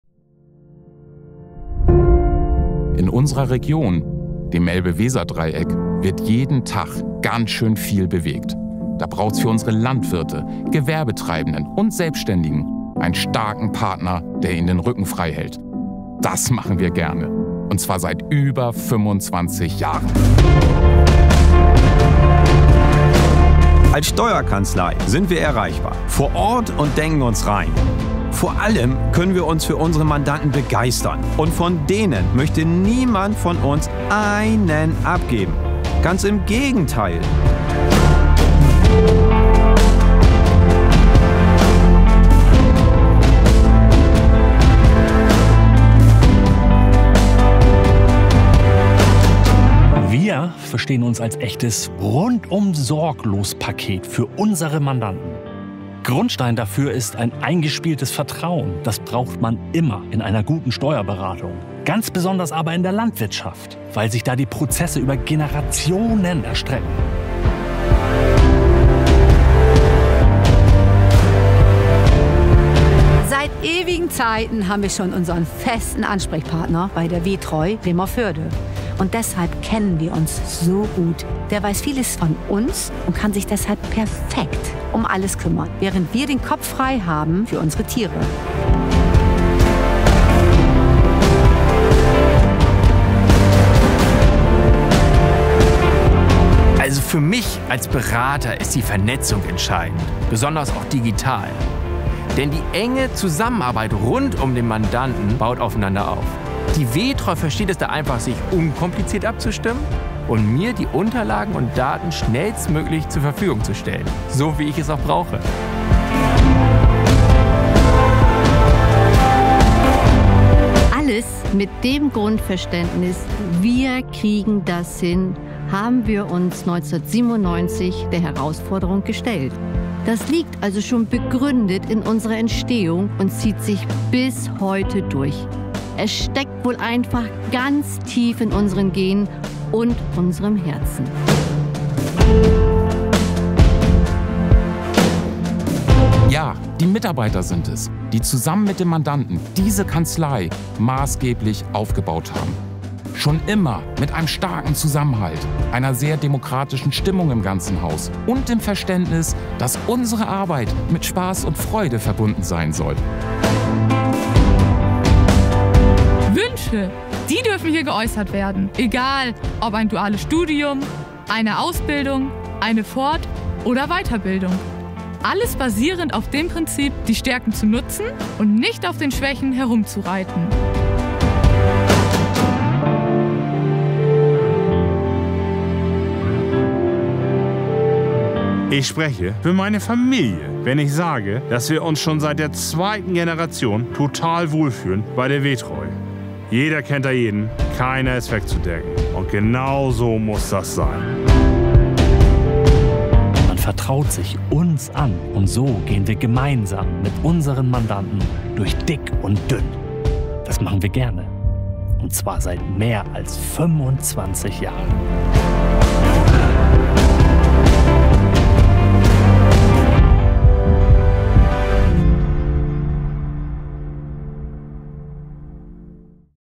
Imagefilm der wetreu Bremervörde: Bestens beraten seit über 25 Jahren.
Vorhang auf für unseren neuen Kanzlei-Film!
Seit über 25 Jahren begeistern wir uns für unsere Mandant*innen im Elbe-Weser-Dreieck! Wir bieten ihnen unser wetreu Rundum-Sorglos-Paket und begleiten sie über Generationen hinweg.

Mit der Medienagentur Johann&Bendix entstand ein Film, der unsere Kanzleiphilosophie in Bilder kleidet.
Unter der Maxime "Wir kriegen das hin!" wurde die wetreu Kanzlei in Bremervörde vor über 25 Jahren gegründet und heute sind es die Kolleg*innen, die mit Freude und ihren persönlichen Stärken für eine harmonische Zusammenarbeit mit der Mandantschaft sorgen.

Der Film ist ein Dankeschön an all unsere Mandant*innen und Mitarbeiter*innen. Sie sind Teil unserer über 25jährigen Geschichte, die sie mit ihrer Zusammenarbeit und Treue maßgeblich mitgeschrieben haben.

wetreu. Bestens beraten.